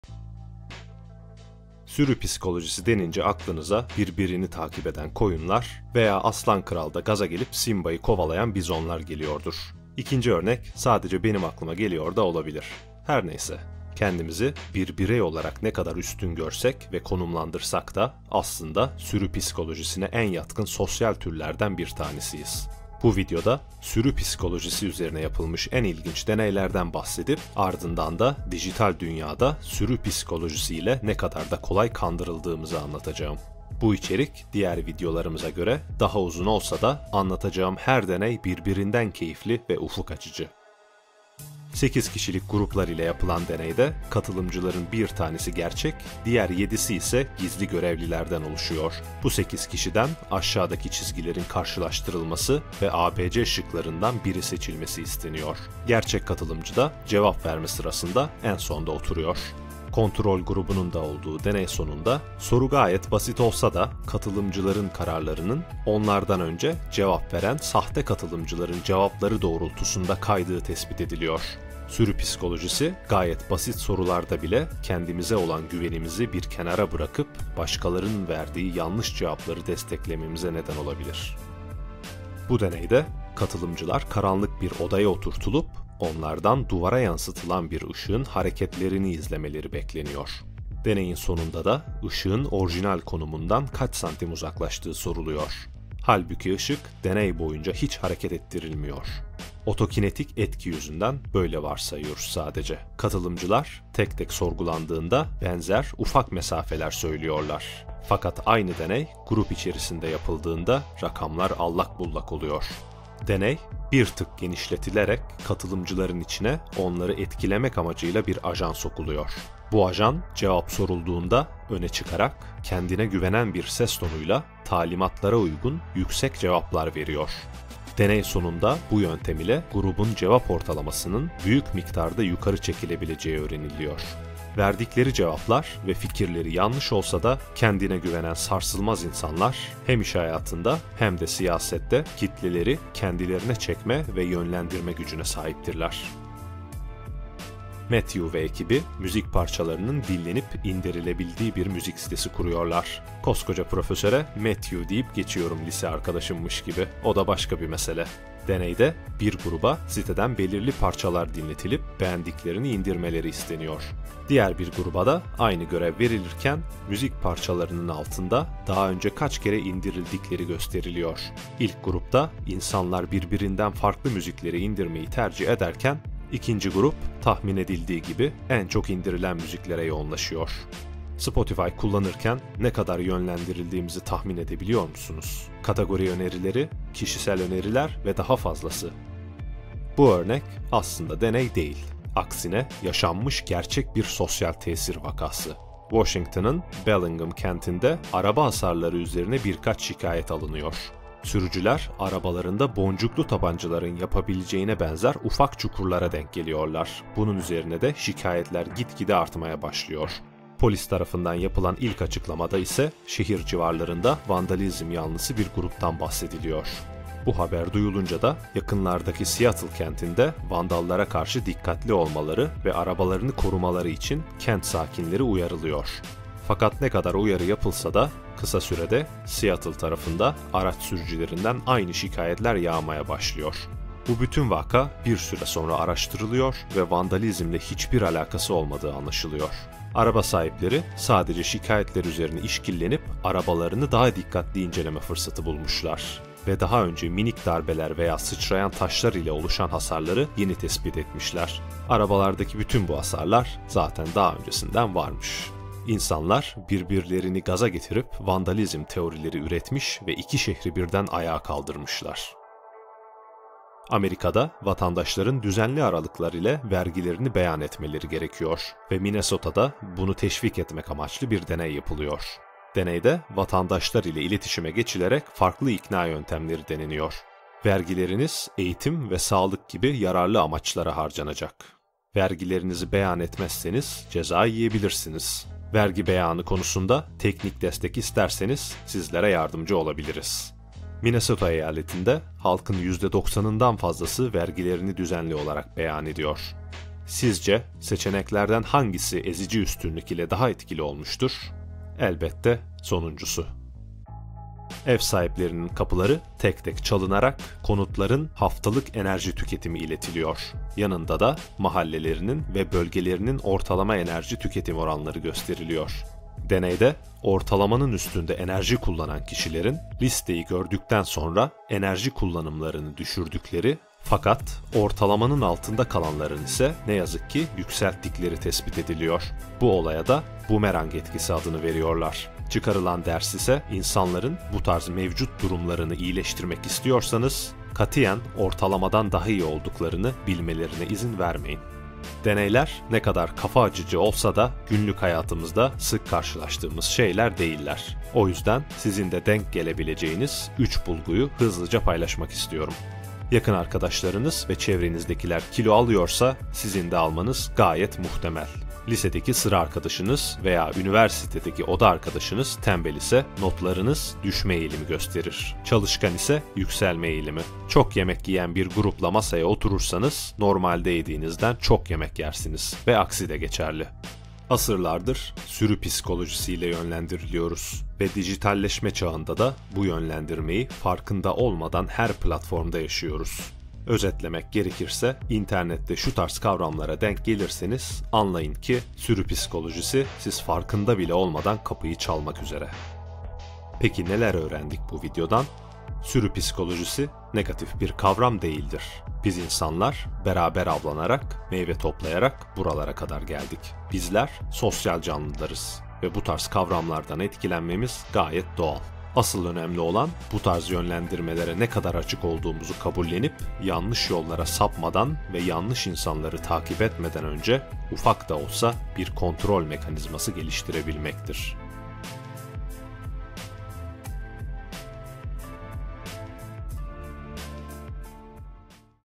Sürü Psikolojisi Deneyleri ve Koyunlaşmak
Sürü psikolojisi denince birbirini kovalayan koyunlar veya aslan kralda gaza gelip Mufasa’yı ezen bizonlar geliyordur aklınıza.

Sadece benim aklıma geliyor da olabilir ikinci örnek...

Kendimizi bireysel olarak ne kadar üstün görsek ve konumlandırsak da aslında sürü psikolojisine en yatkın türlerden biriyiz.

İçerik diğerlerine göre biraz uzun fakat her deney birbirinden keyifli ve ufuk açıcı.

Sürü Psikolojisi Deneyleri

Asch Deneyi — Solomon Asch (1951)
Sekiz kişilik deneyde, katılımcıların bir tanesi gerçek diğer yedisi ise gizli görevliler olduğunu düşünün. Bu sekiz kişiden aşağıdaki çizgilerin karşılaştırılması ve A-B-C şıklarından biri seçilmesi isteniyor. Gerçek katılımcı da cevap verme sırasında en sonda oturuyor. Kontrol grubunun da olduğu deney sonunda da, soru gayet basit olsa da katılımcıların kararlarının ondan önce cevap veren sahte katılımcıların cevapları doğrultusunda kaydığı tespit ediliyor. Sürü psikolojisi, gayet basit sorularda bile kendimize olan güvenimizi bir kenara bırakıp, başkalarının verdiği yanlış cevapları desteklememize neden olabilir.

Sherif Deneyi — Muzafer Sherif (1935)
Bu deneyde katılımcılar karanlık bir odaya oturtulup, onlardan duvara yansıtılan bir ışığın hareketlerini izlemeleri bekleniyor. Deneyin sonunda da ışığın orijinal konumundan kaç santim uzaklaştırıldığı soruluyor. Halbuki ışık, deney boyunca hiç hareket ettirilmiyor, “otokinetik etki” yüzünden böyle varsayıyoruz sadece.

Katılımcılar tek tek sorgulandığında benzer mesafeler söylüyorlar fakat aynı deney grup içerisinde yapıldığında rakamlar allak bullak oluyor. Sherif deneyi bir tık geliştirerek katılımcıların içine, onları etkilemek amacıyla bir kişi sokuyor. Bu kişi cevap sorulduğunda öne çıkarak, kendine güvenen bir ses tonuyla talimatlara uygun cevaplar veriyor. Deney sonucunda, bu sayede tüm gurubun cevap ortalamasını büyük miktarda yukarı veya aşağı çekilebileceği öğreniliyor.

Verdikleri cevap veya fikirleri yanlış olsa da, kendine güvenen sarsılmaz insanlar, hem iş hayatında hem siyasette kitleleri kendine çekme ve yönlendirme gücüne sahiptirler.

Ama biz koyun değiliz değil mi, çok zeki insanlarız, böyle kolay yönlendirilmeyiz değil mi? İkinci Dünya Savaşı’nda Hitler’in komutasında çalışan askerlerin belgesel ve röportajlarına bir göz atın derin. Neyse bu bambaşka bir konu, deneylere devam edelim biz en iyisi…

Musiclab — Matthew Salganik (2006)

Matthew ve ekibi, müzik parçalarının dinlenip indirilebildiği bir müzik sitesi kuruyorlar. Koskoca profesöre Matthew deyip geçiyorum lise arkadaşım gibi, o da başka bir mesela. Deneyin bir grubuna belirli parçalar dinletilip katılımcılardan beğendiklerini indirmeleri isteniyor. Diğer bir gruba da aynı görev verilirken, müzik parçalarının altında daha önce kaç kere indirdikleri gösteriliyor. İlk grupta insanlar birbirinden farklı müzikleri indirmeyi tercih ederken, ikinci grup tahmin edildiği gibi en çok indirilen müziklere yoğunlaşıyor. Bu arada deneyde kullanılan müzikler popüler değil ve daha önce katılımcıların muhtemelen hiç duymadığı şarkılardan oluşuyor.

Spotify kullanırken ne kadar yönlendirildiğimizi tahmin edebiliyor musunuz? Kategori önerileri, kişisel öneriler ve daha fazlası, bir düşünün. Bu yönlendirmelerin negatif şeyler olduğunu söylemiyorum elbette, fakat kendimizi çelik zırhlı sarsılmaz süvariler görmekten vazgeçelim istiyorum.

Seattle Windshield Pitting Epidemic

Bu aslında deney değil ve yaşanmış gerçek bir sosyal tesir vakası, okurken de yarıldığımı söyleyebilirim. Washington’ın Bellingam kentinde, araba hasarları üzerine bir kaç şikayet alınıyor. Arabalarda boncuklu tabancaların yapabileceğine benzer ufak çukurlara denk geliyor sürücüler, bunun üzerine de şikayetler git gide artmaya başlıyor. Güvenlik tarafında yapılan açıklamada ise şehir civarlarında vandalizm yanlısı bir gruptan bahsediliyor. Bu olay büyüyünce de, yakınlardaki Seattle kentinde vandallara karşı dikkatli olmaları ve arabalarını korumaları için kent sakinleri uyarılıyor. Fakat ne kadar uyarı yapılsa da kısa sürede Seattle tarafında araç sürücülerinden şikayetler yağmaya başlıyor.